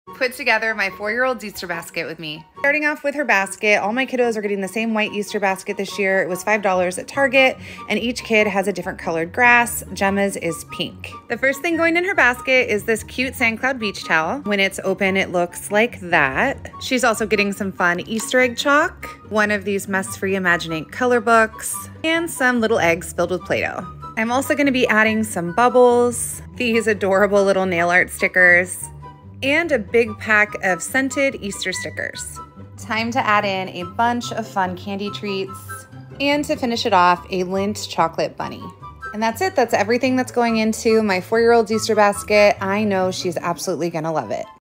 What I Got My Kids For EASTER! 🐰
Let's make an Easter basket for my 4 year old!